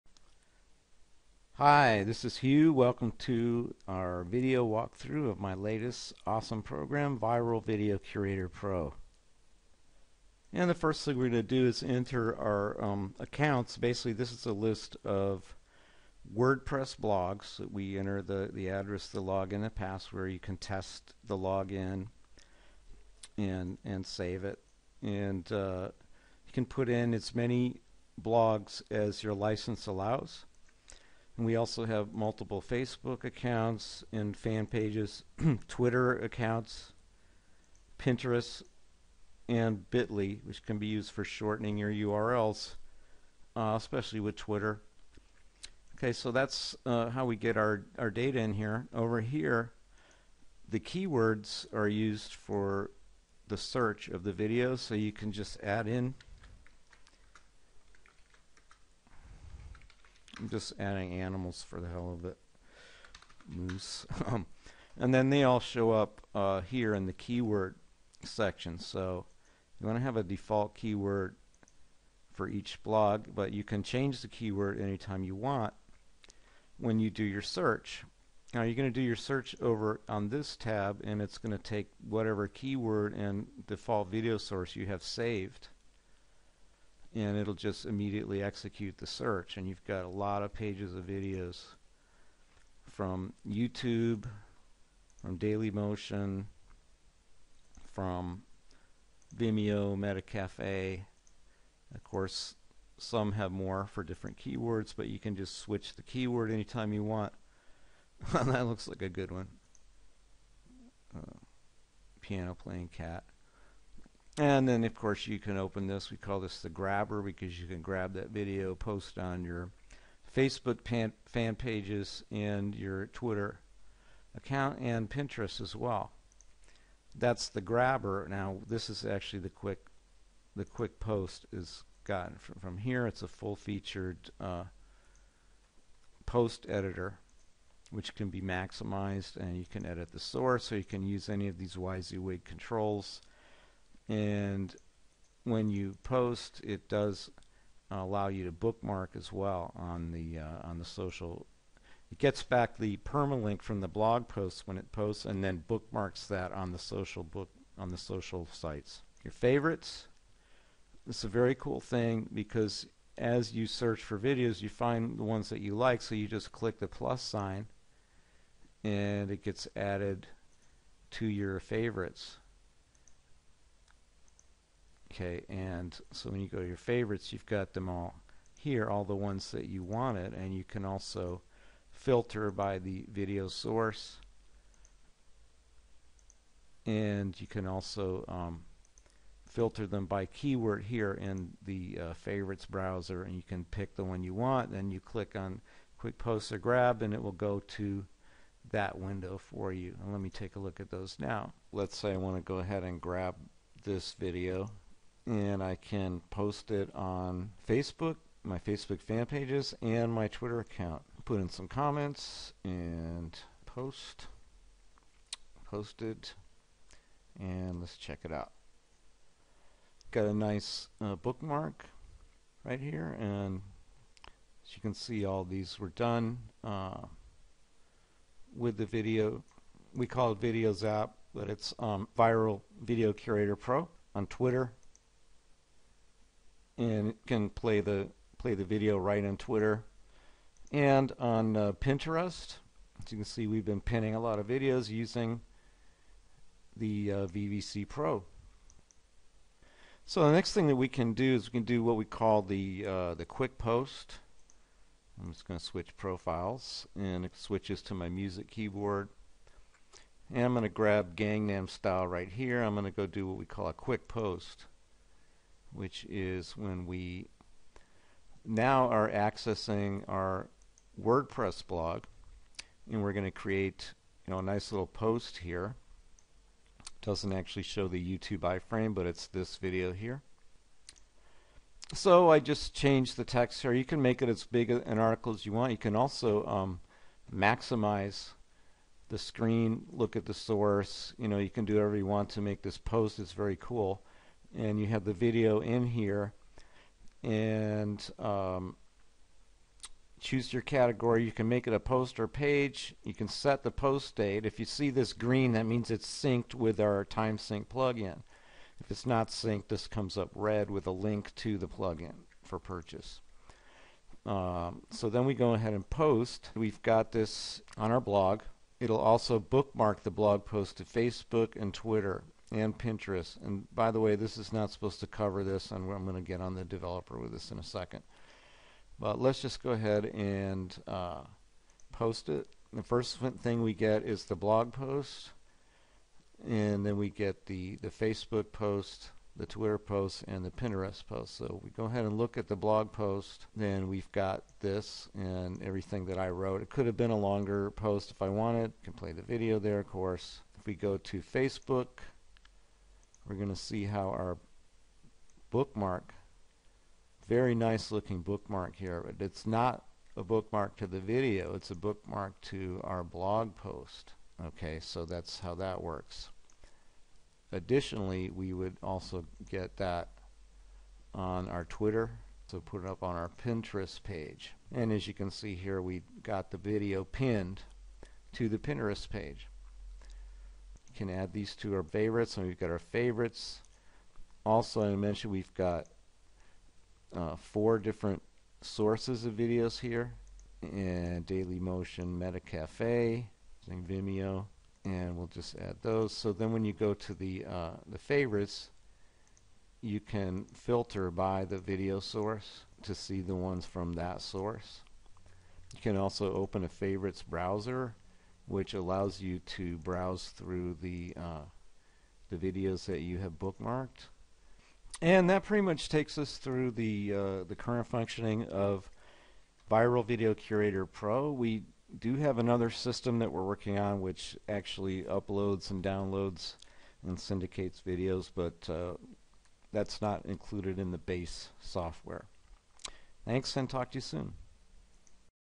Viral Video Curator Pro initial Walkthrough
This is a walkthrough of the functionality of Viral Video Curator Pro which is an all purpose tool for searching, collecting and posting of videos to your blog, facebook twitter and pinterest pages. VVC Pro additionally has a social bookmarking feature to drive traffic to your blog.